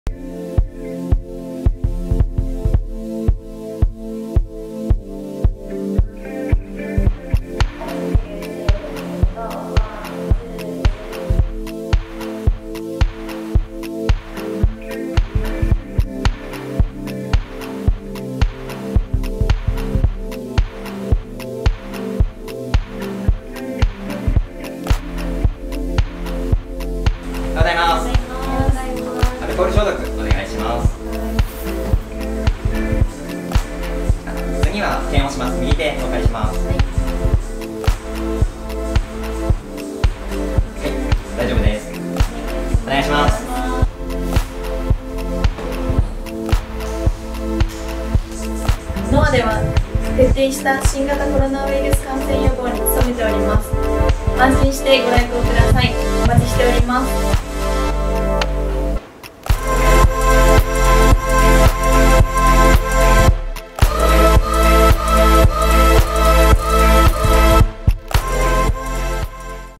【新型コロナウィルス感染対策】@NOA Dance School
お客様と講師の安心安全を守るため以下の感染予防対策を行ってまいります。

【１】
参加人数を制限し、十分なスペースを設けてレッスンを受けていただきます。

【２】
全スタジオ内の強制吸排気設備は充分に強力なものを使用しており
常時吸排気ファンにより空気の入れ替えを行っております。
更にストレッチ時にはスタジオの窓も開けるなどこまめに換気をします。
エアコンフィルタ等の清掃も徹底しております。

【３】
スタジオ内のコロナ感染予防のため、
全校に空間除菌装置を設置します。
電解の力により効率よく二酸化塩素を発生させ、
ウィルス除去、除菌、防カビ効果があります。

【４】
消毒の徹底とスタッフのマスクの着用、出勤時の検温、体調管理を徹底します。
対面による直接の飛沫感染を防ぐ為、
受付にはフロントアクリルを設置させていただきます。

【５】
お客様には受付にて体調に関するチェックシートの記入と
非接触体温計を用いた検温をさせて頂いております。
発熱や風邪等の症状がみられる方はレッスンの受講は
お断りさせていただいております。

【６】
スタジオ内はもちろん、全店にクレベリン等を
設置し受付等の空間除菌にも努めております。

【７】
レンタル品、備品、ドアノブや手すりなど
不特定多数の方が触れるものに対してはアルコール等で
徹底的に消毒しています。

【８】
入館時にはお客様にも消毒液よる手指の消毒及び、
手洗いの徹底にて感染予防にご協力をお願いしております。

お客様には以下のご協力をお願いします。

●消毒液による手指の消毒及び、手洗いの徹底にて感染予防にご協力をお願いします。

●ご来校時、またスタジオ内でのマスク等の着用をお願いいたします。

●スタジオでの滞在時間は最小限でお願いします。

●スタジオ・ロッカー・ロビー・廊下等をご利用の際は、お客様相互の間隔にご配慮下さい。
※ロッカールームのご利用は10分以内とさせていただきます。